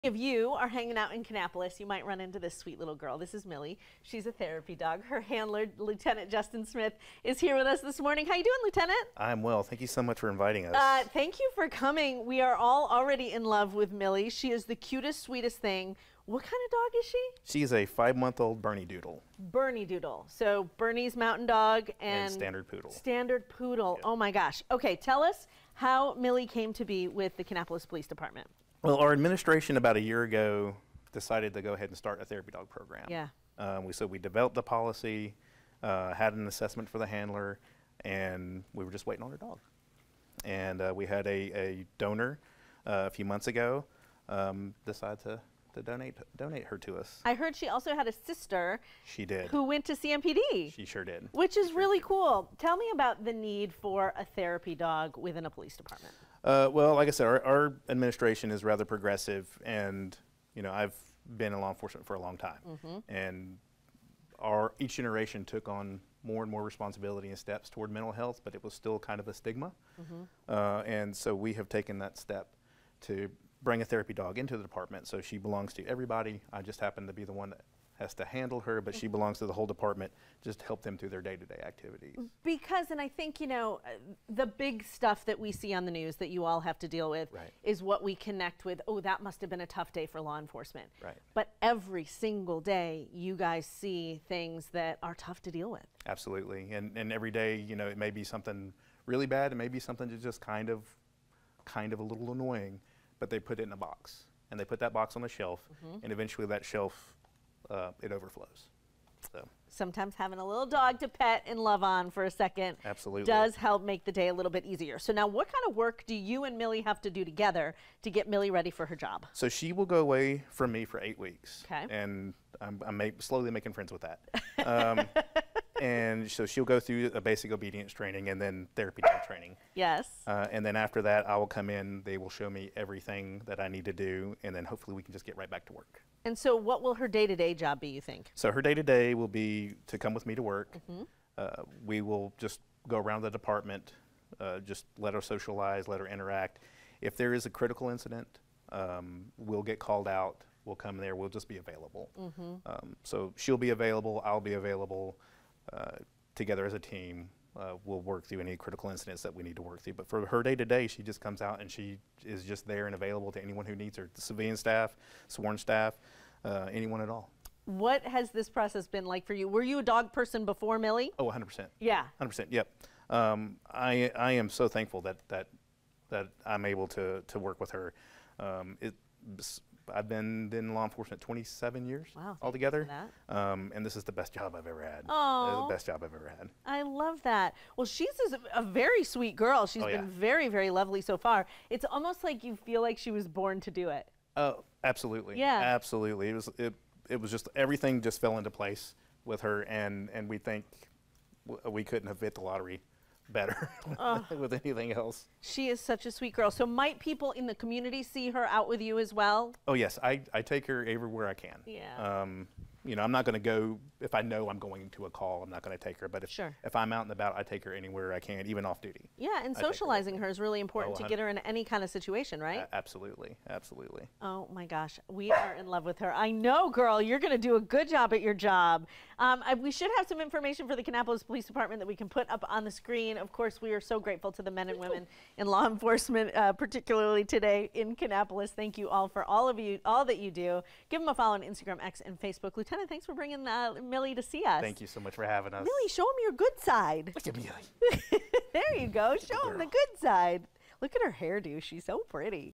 Meet Kannapolis PD’s newest member
KPD Lieutenant introduces the department’s new therapy dog.